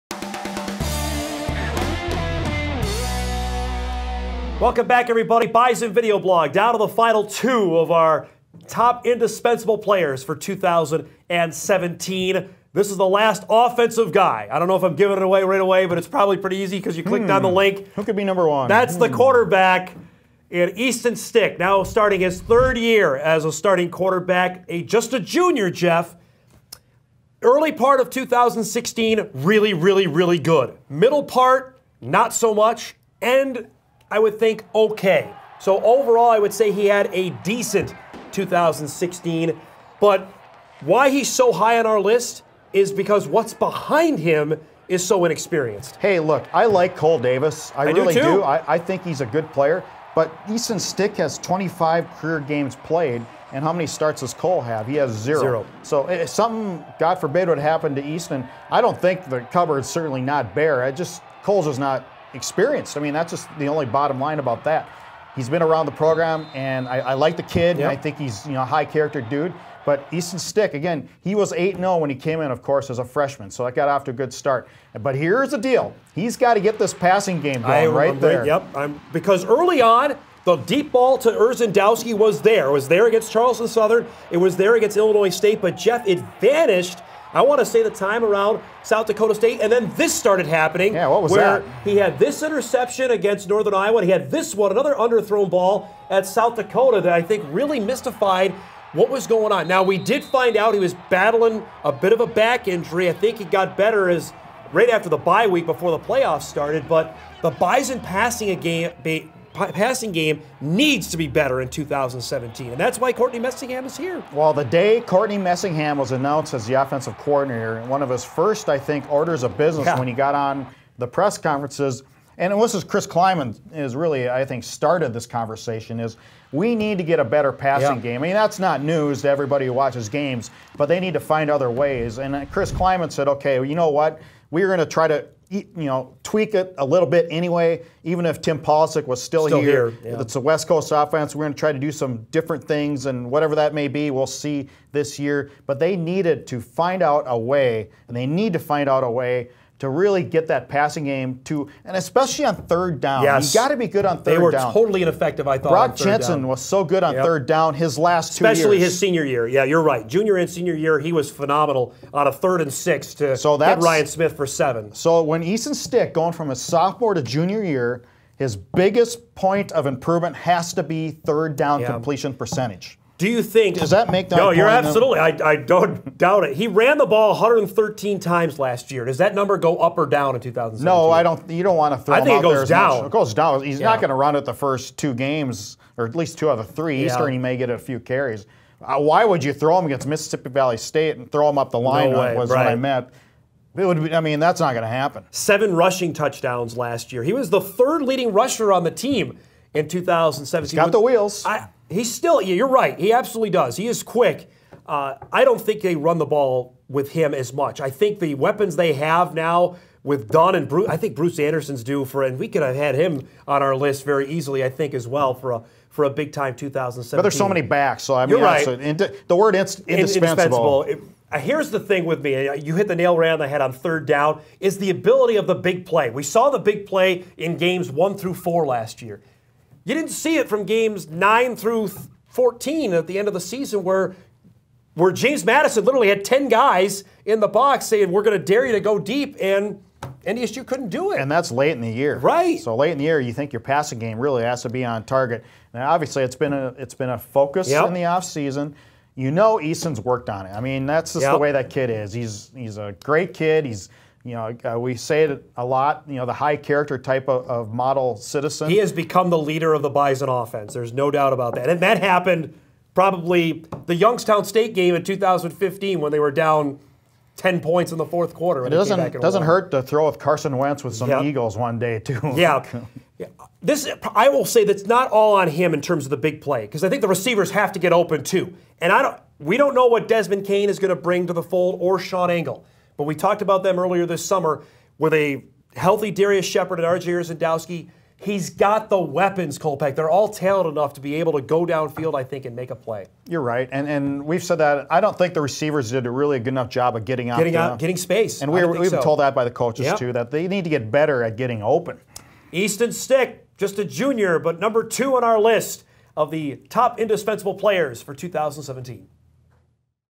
Top 7 Most Indispensable Players in 2017: Easton Stick
The final countdown of our summer video series rolls on as Jeff and Dom take a look at the "Top 7 Most Indispensable Players in 2017."

You can also follow the NDSUSeasonOpenerCountdown on Twitter and Facebook, just use that hashtag to follow along. We hope to quench your football thirst until fall camp begins in late July.